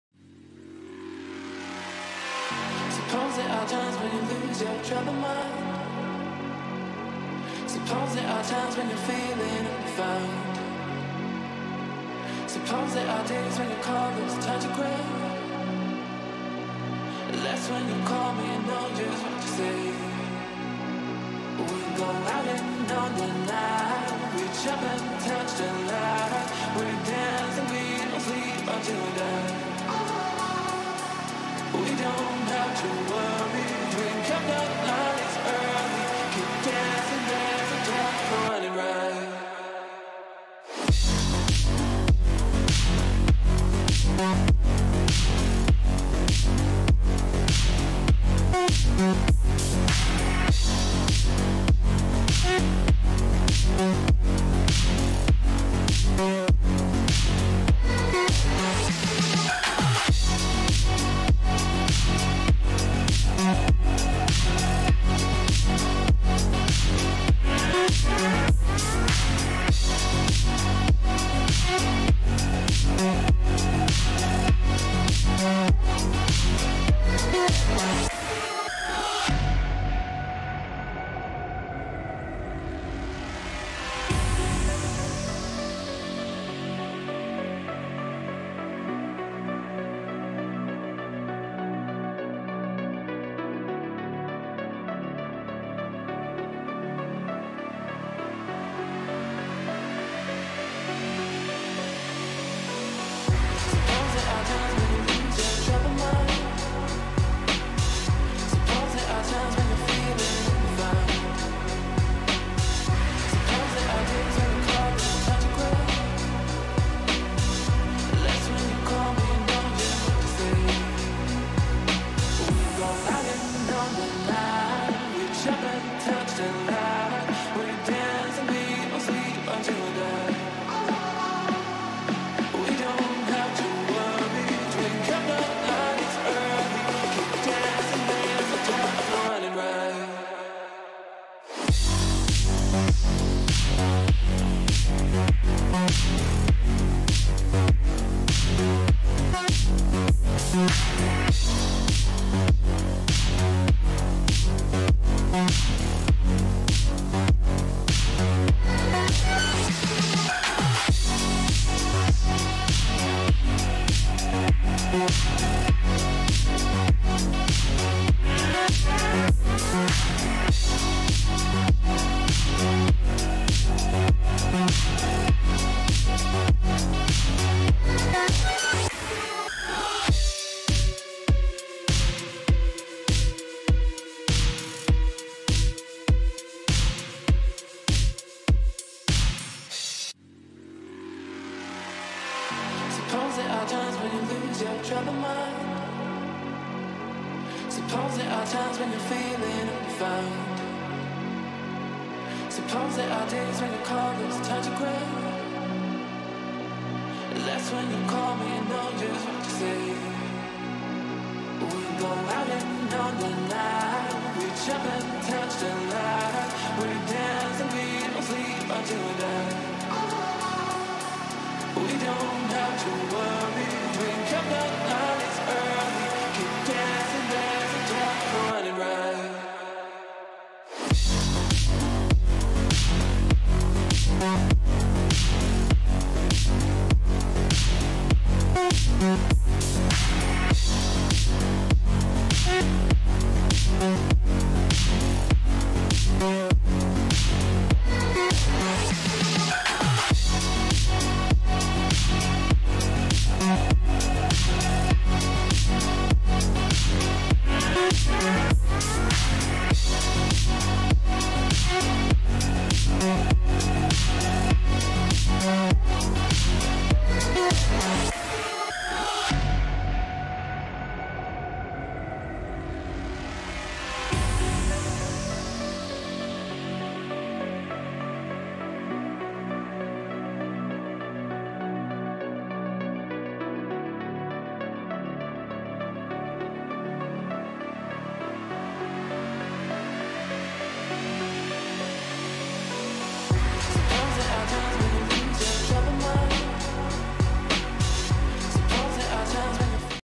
GTA VI Grand Theft Auto 6 Cars
GTA VI Grand Theft Auto 6 concept cars! We are eager to see all the concept cars we will see in GTA VI! Here are some examples of what we wish to see! If you enjoyed this video check this one out